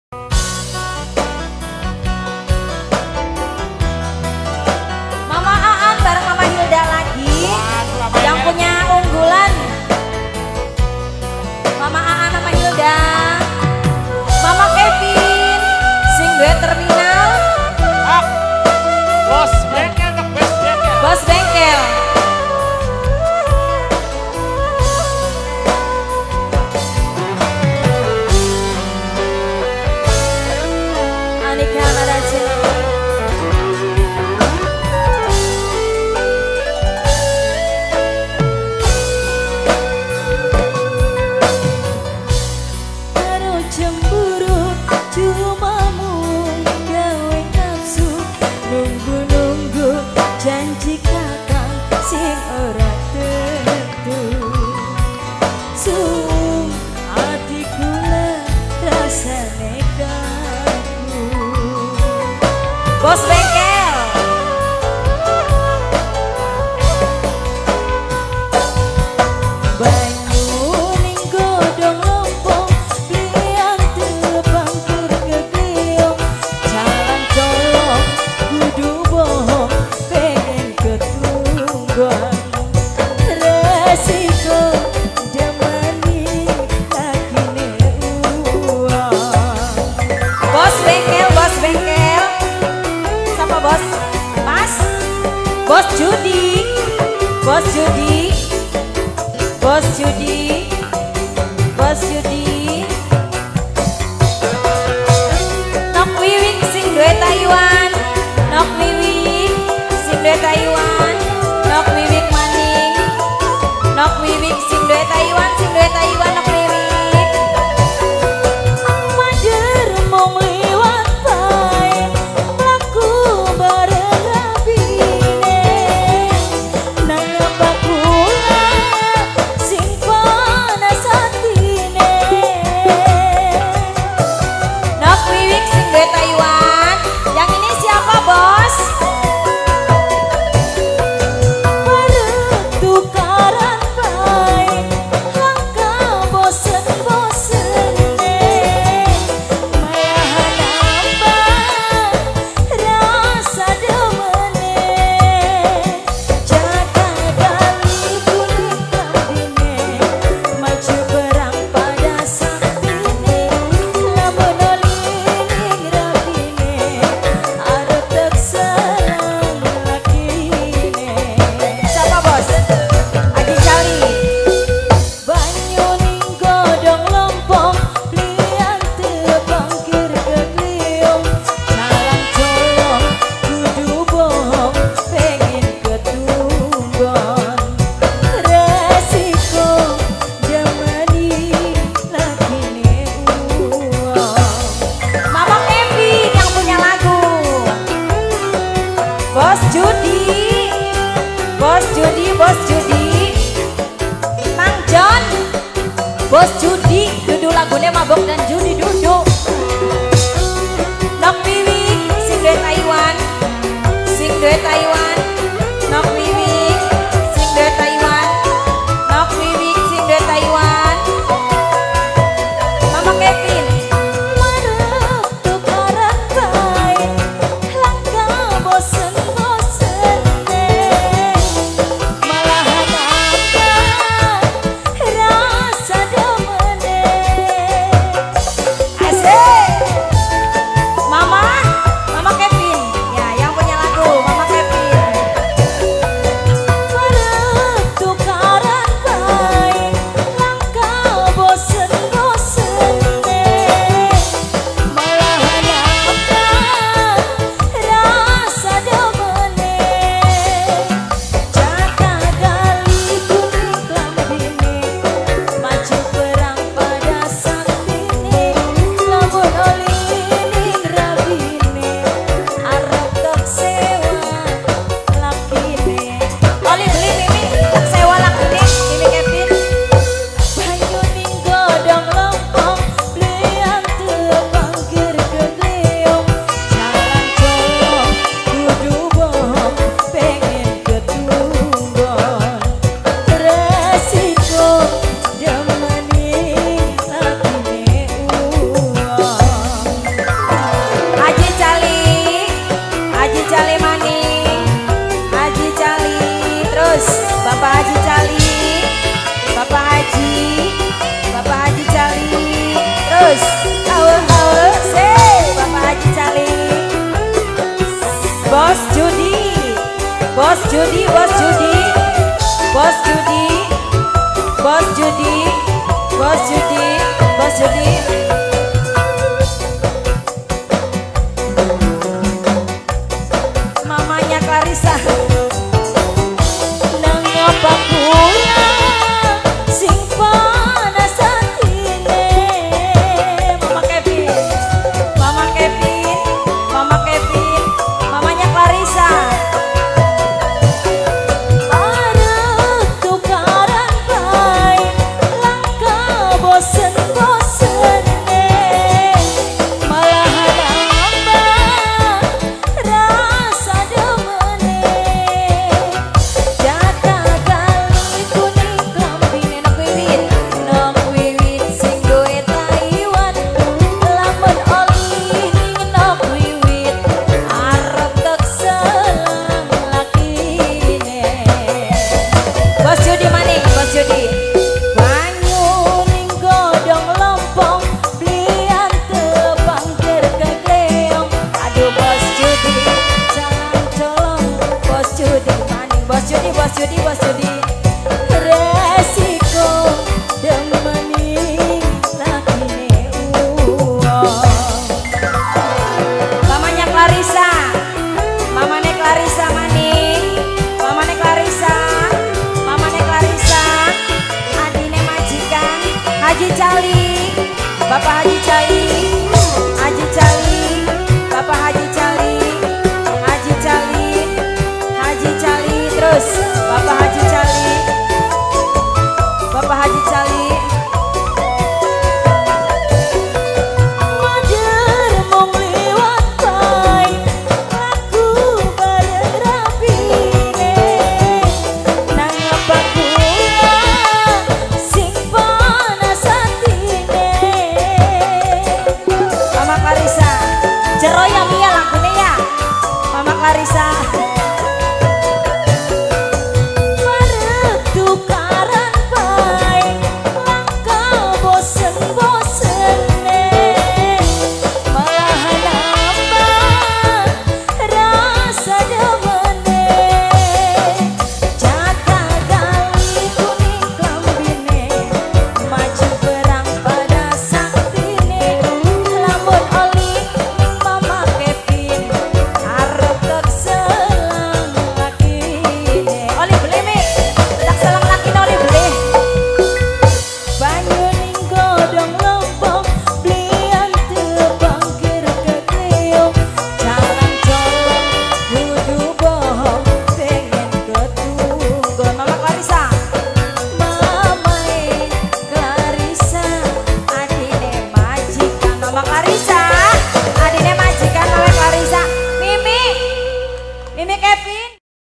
AIU KARLINA TEKSSELANG LAKINE ANICA NAD SIANG 07 JUNI 2021 DESA TERUSAN SINDANG INDRAMAYU
DOKUMENTASI BAPAK ROKIDIN/IBU DEVIT
DALAM RANGKA WALIMATUL KHITAN ALLIAN ARDIANSYAH
SENIN MALAM SELASA 07 JUNI 2021
SUKADEDEEL RT.17 RW.06 DESA TERUSAN
BLOK BALAI DESA PECANTILAN
KECAMATAN SINDANG KABUPATEN INDRAMAYU